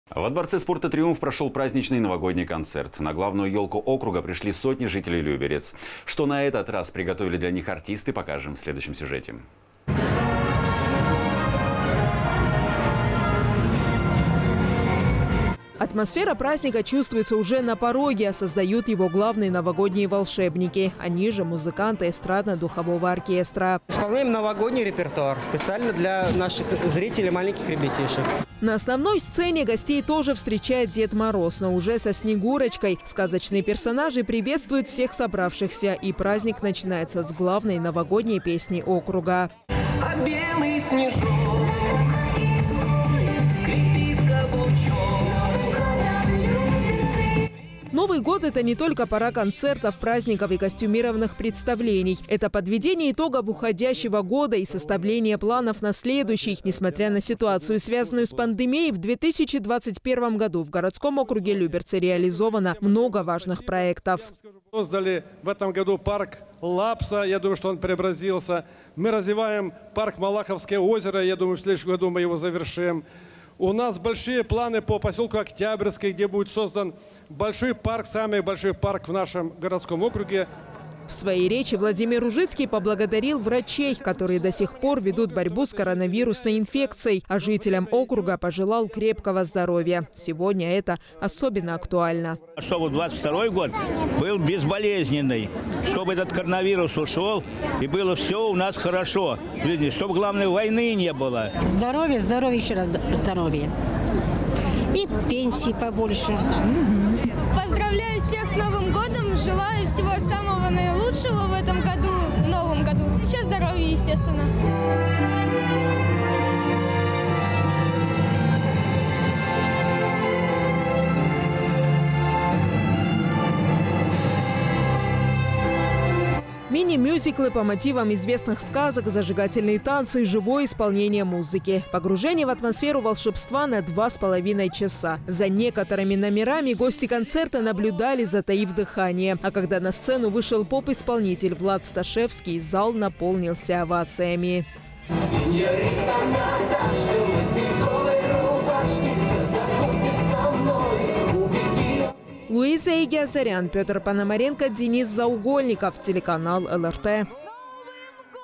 С Новым годом, Люберцы!
Во Дворце спорта «Триумф» прошел праздничный новогодний концерт. На главную елку округа пришли сотни жителей Люберец. Что на этот раз приготовили для них артисты, покажем в следующем сюжете.

Атмосфера праздника чувствуется уже на пороге. А создают его главные новогодние волшебники. Они же – музыканты эстрадно-духового оркестра.

Илья Глущенко, руководитель духового оркестра:
«Исполняем новогодний репертуар. Специально для наших зрителей – маленьких ребятишек».

На основной сцене гостей тоже встречает Дед Мороз, но уже со Снегурочкой. Сказочные персонажи приветствуют всех собравшихся, и праздник начинается с главной новогодней песни округа.

Новый год – это не только пора концертов, праздников и костюмированных представлений. Это подведение итогов уходящего года и составление планов на следующий. Несмотря на ситуацию, связанную с пандемией, в 2021 –ом году в городском округе Люберцы реализовано много важных проектов.

Владимир Ружицкий, глава г.о. Люберцы:
«Мы создали в этом году парк Лапса, я думаю, что он преобразился. Мы развиваем парк «Малаховское озеро», я думаю, в следующем году мы его завершим. У нас большие планы по поселку Октябрьский, где будет создан самый большой парк в нашем городском округе».

В своей речи Владимир Ружицкий поблагодарил врачей, которые до сих пор ведут борьбу с коронавирусной инфекцией. А жителям округа  пожелал крепкого здоровья. Сегодня это особенно актуально.

Виктор Трантров, житель г.о. Люберцы:
«Чтобы 22 –й год был безболезненный, чтобы этот коронавирус ушел, и было все у нас хорошо в жизни, чтобы, главное, войны не было».

Людмила Демьяненко, жительница г.о Люберцы:
«Здоровья, здоровья и еще раз здоровья. И пенсии побольше».

Матвей Кузнецов, житель г.о. Люберцы:
«Поздравляю всех с Новым годом, желаю всего наилучшего в Новом году. Все. А, ну еще здоровья, естественно».

Мини-мюзиклы по мотивам известных сказок, зажигательные танцы и живое исполнение музыки. Погружение в атмосферу волшебства на 2 с половиной часа. За некоторыми  номерами гости концерта наблюдали, затаив дыхание. А когда на сцену вышел поп-исполнитель Влад Сташевский, зал наполнился овациями.

Луиза Егиазарян. Петр Пономаренко. Денис Заугольников.